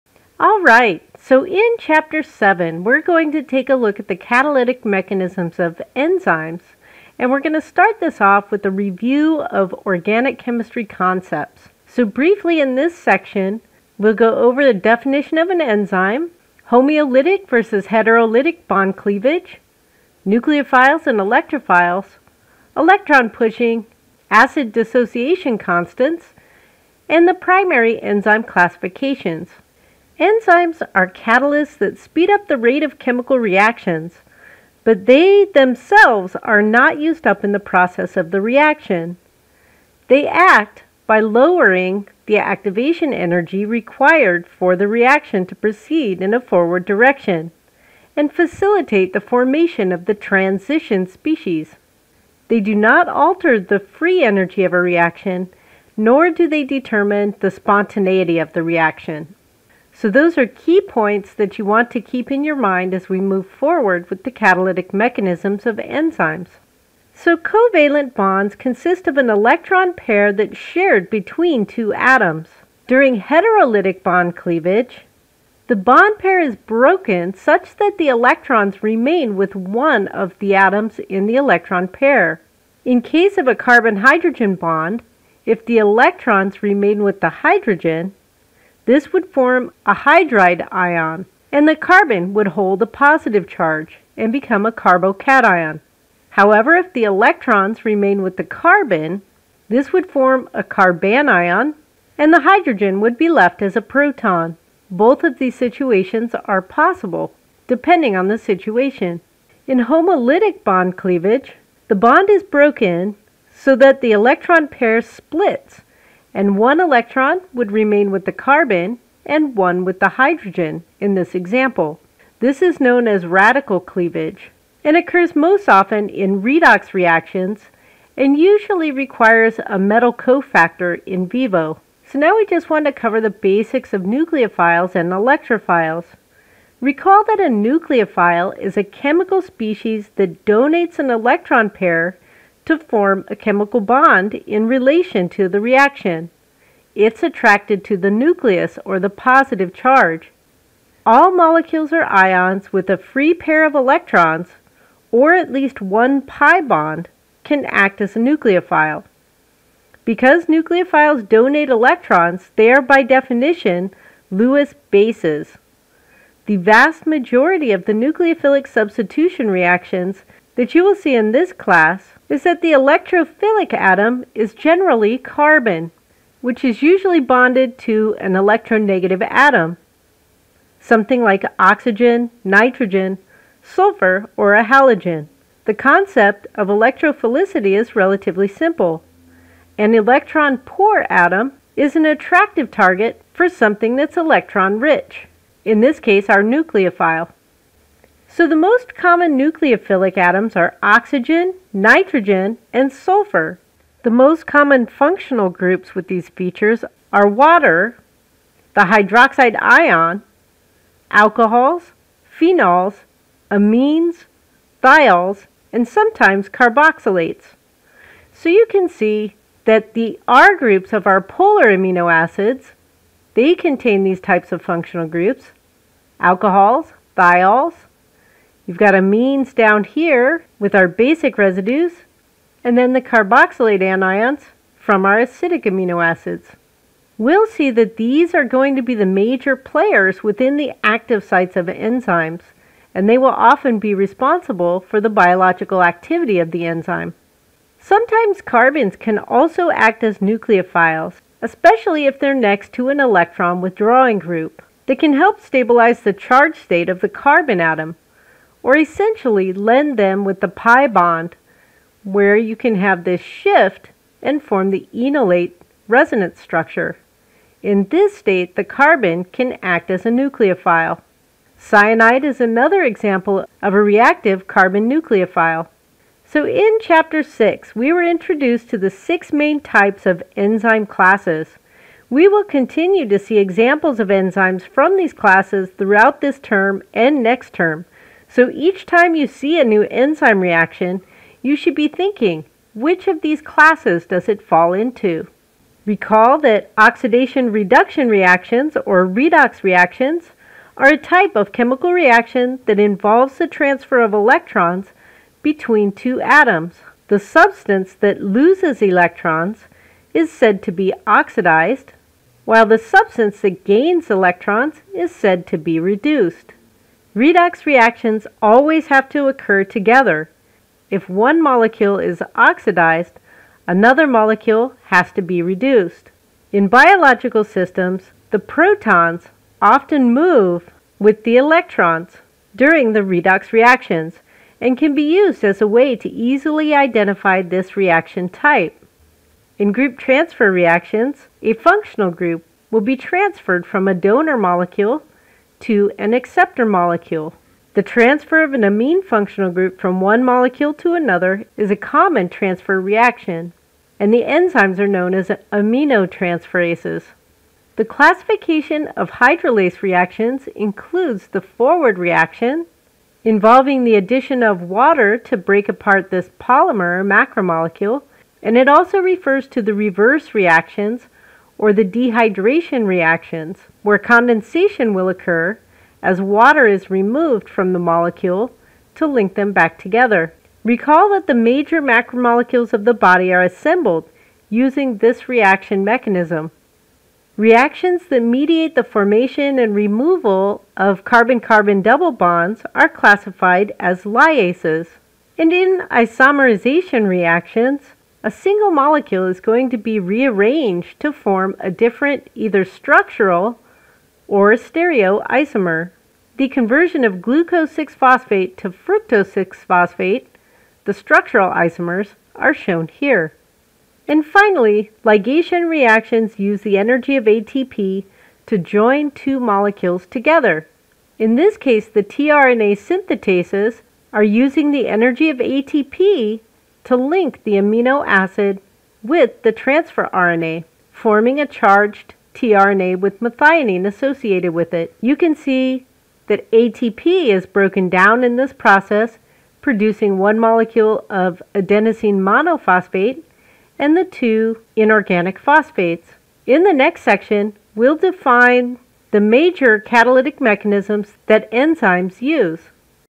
CH450 Chapter 7.1 Review for Enzyme Reactions
This lecture reviews basic organic chemistry concepts needed to study the catalytic strategies used by enzymes to facilitate chemical reactions. Concepts covered include: homolytic and heterolytic bond cleavage, nucleophiles and electrophiles, and primary enzyme classifications.